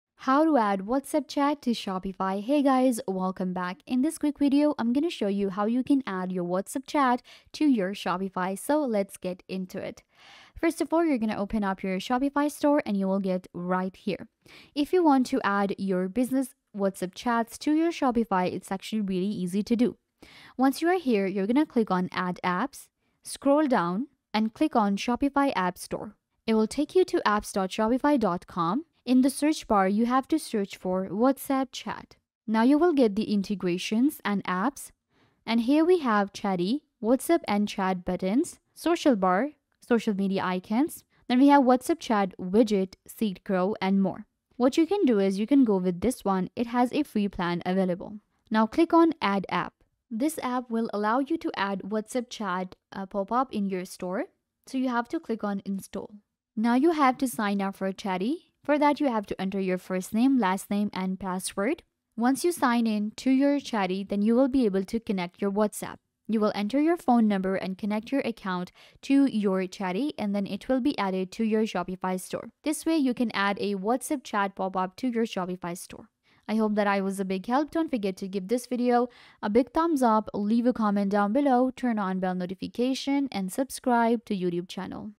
How To Add WhatsApp Chat To Shopify 2026 (EASY & FAST)
In this video we show you How To Add WhatsApp Chat To Shopify. It is really easy to do and learn to do it in just a few minutes by following this super helpful tutorial to get a better understanding of everything.